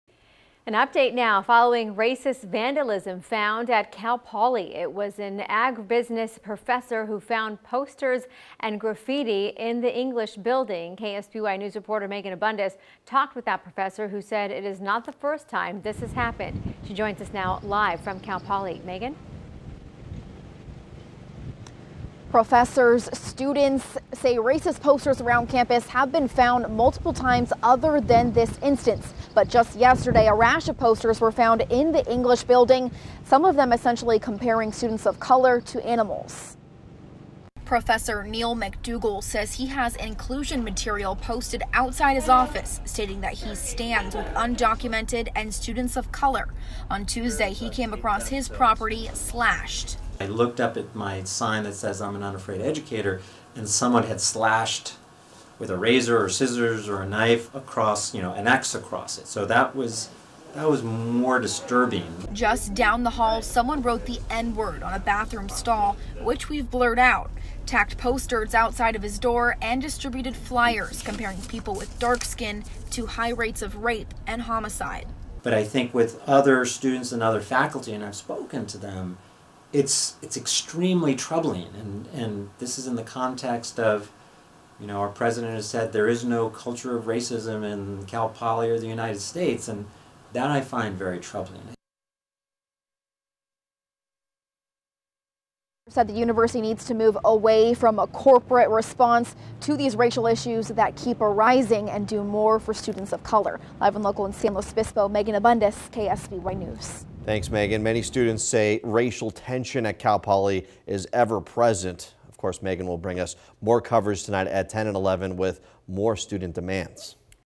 Racist vandalism discovered in Cal Poly English building, professor says
A Cal Poly professor shared online Tuesday what may be the latest in a string of racist incidents straining the Cal Poly community.